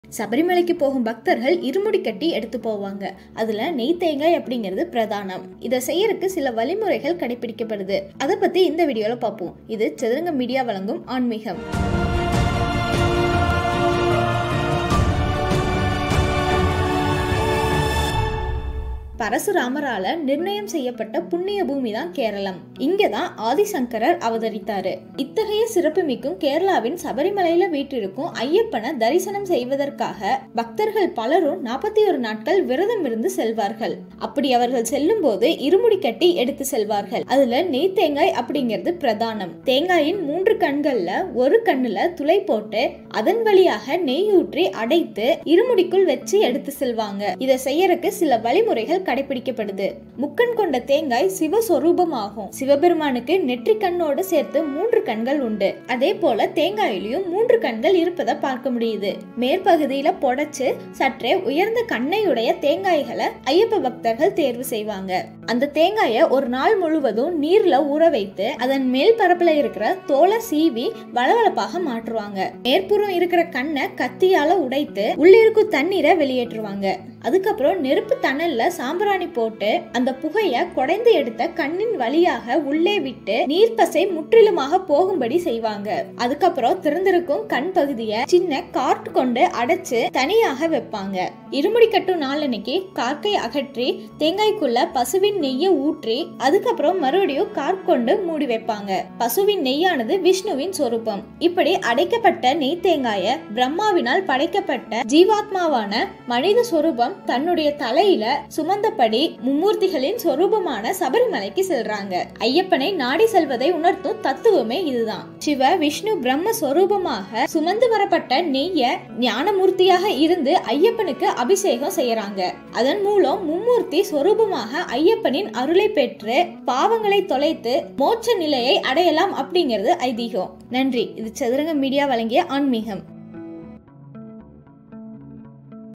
சபரிமலை நெய் தேங்காவின் உள்ள உண்மை காரணம் என்ன ?
பரசுராமரால் நிர்மாணம் செய்யப்பட்ட புண்ணிய பூமி, கேரளம். இங்குதான் ஆதிசங்கரர் அவதரித்தார். இத்தகைய சிறப்புமிக்க கேரளாவின் சபரிமலையில் வீற்றிருக்கும் ஐயப்பனை தரிசனம் செய்வதற்காக, பக்தர்கள் பலரும் 41 நாட்கள் விரதம் இருந்து செல்வார்கள். அப்படி அவர்கள் செல்லும்போது இருமுடி கட்டி எடுத்துச் செல்வார்கள். அதில் நெய் தேங்காய் என்பது பிரதானம். தேங்காயின் மூன்று கண்களில் ஒரு கண்ணில் துளைபோட்டு, அதன் வழியாக நெய்யை ஊற்றி அடைத்து, இருமுடிக்குள் வைத்து எடுத்துச் செல்வார்கள். இதைச் செய்வதற்கு சில வழிமுறைகள் கடைப்பிடிக்கப்படுகின்றன.